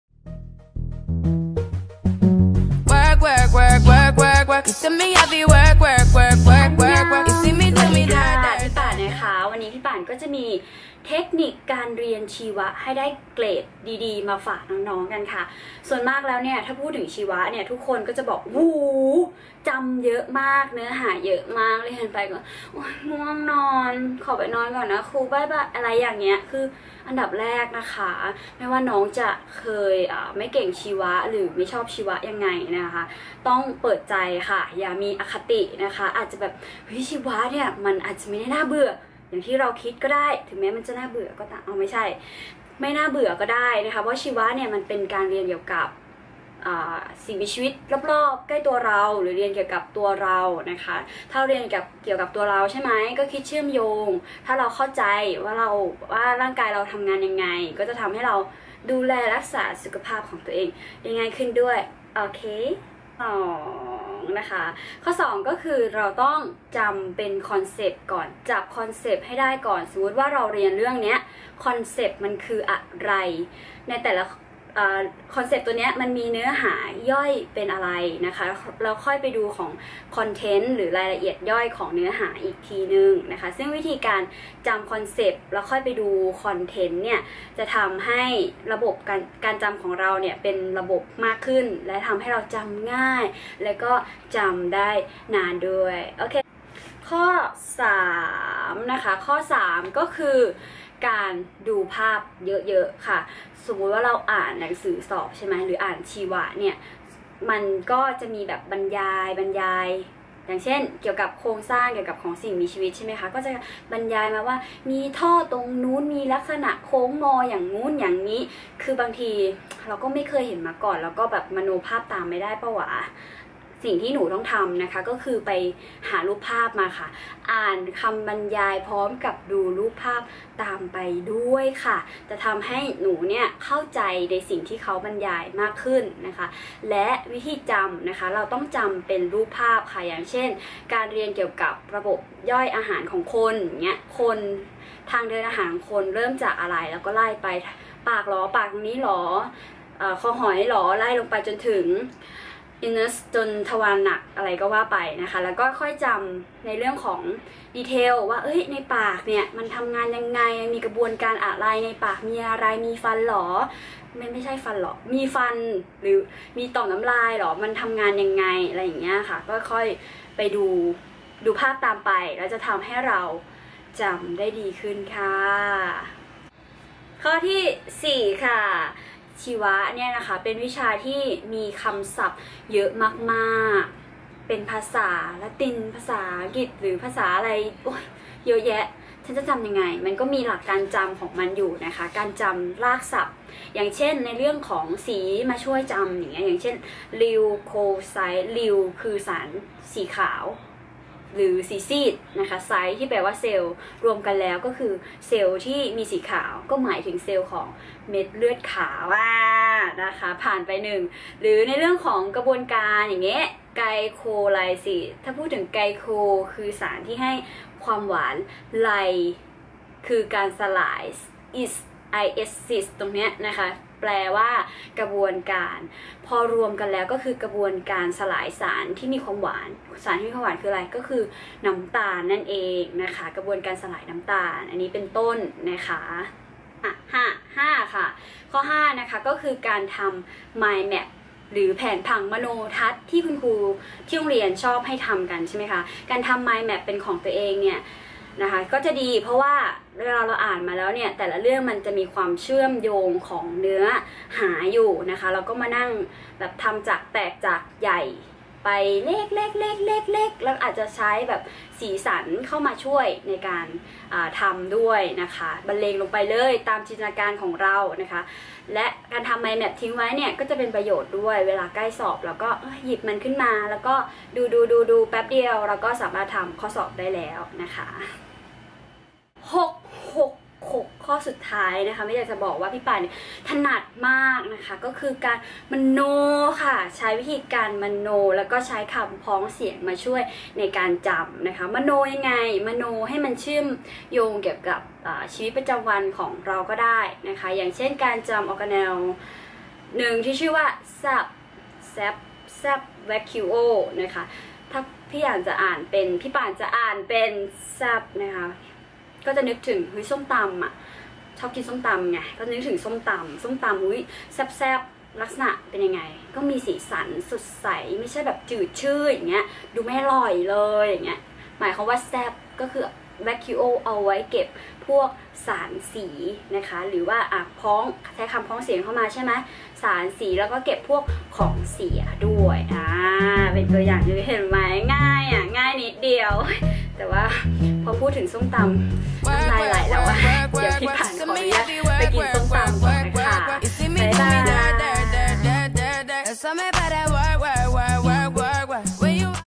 เรียนชีวะอย่างไร ให้ได้คะแนนดี
เรียนชีวะอย่างไรให้ได้คะแนนดีเรียนชีวะอย่างไรให้ได้คะแนนดี
1. เปิดใจไล่อคติ
2. เข้าใจConcept ก่อนเติม Content
3. ดูภาพประกอบ
4. วิเคราะห์รากศัพท์
5. ทำ My map
6. จินตนาการและการใช้คำพ้องเสียง
7.หัดทำโจทย์ทำแบบฝึกหัด